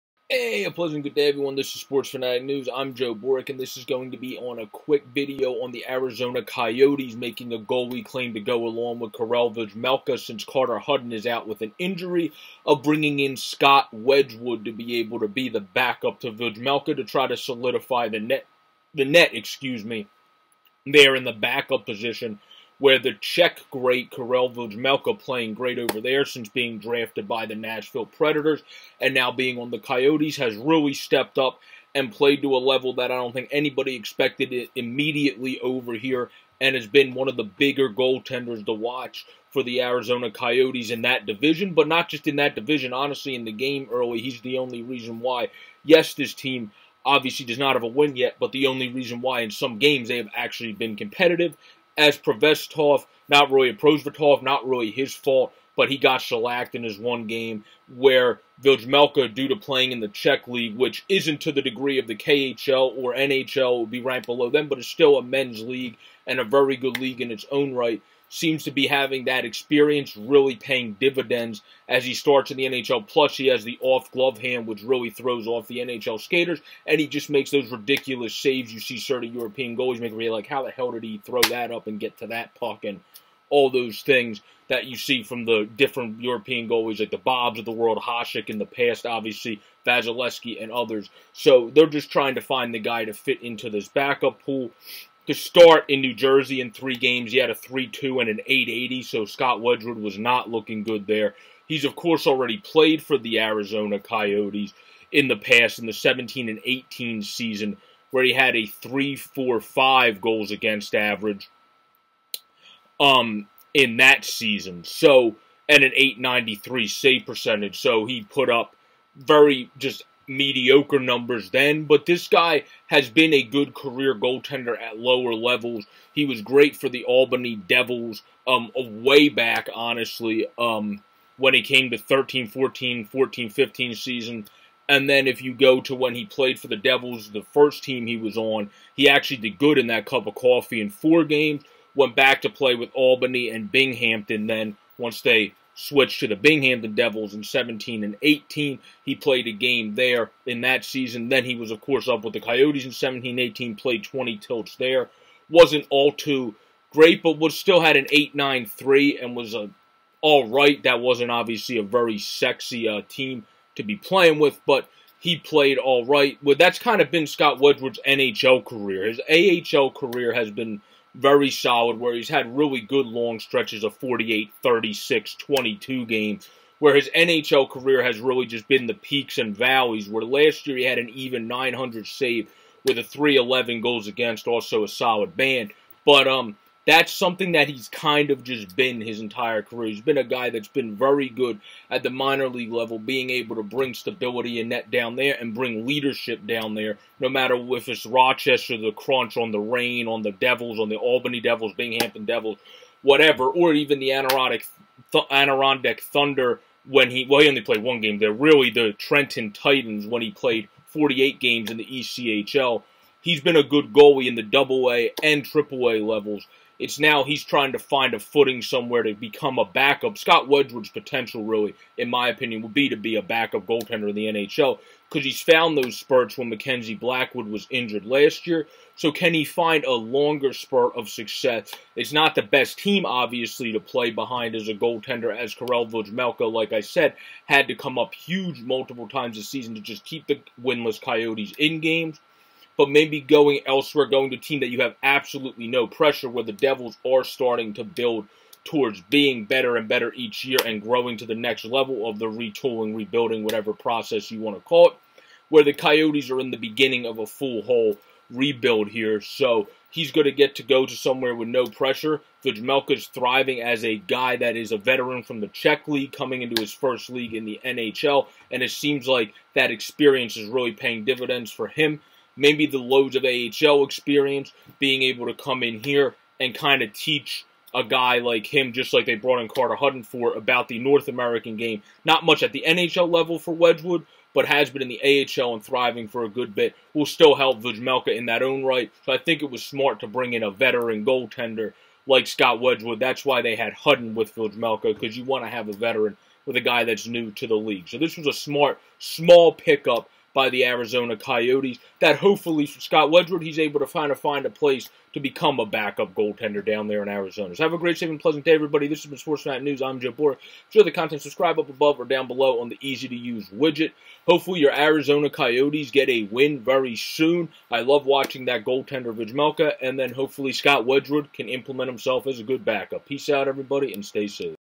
Coyotes Claim Scott Wedgewood to try to stabilize the Backup Goalie Position
The Arizona Coyotes make a small waiver claim to try to solidify their backup goaltender position while Carter Hutton is out. The Player they acquired is AHL Veteran Scott Wedgewood who has had success with multiple teams at the AHL level and some streaks of Sucess with the Devils as well as the Coyotes in the NHL. Wedgewood now returns to the Coyotes to try to stabilize himself as an NHL backup. I will be rooting for him. Appreciate the support all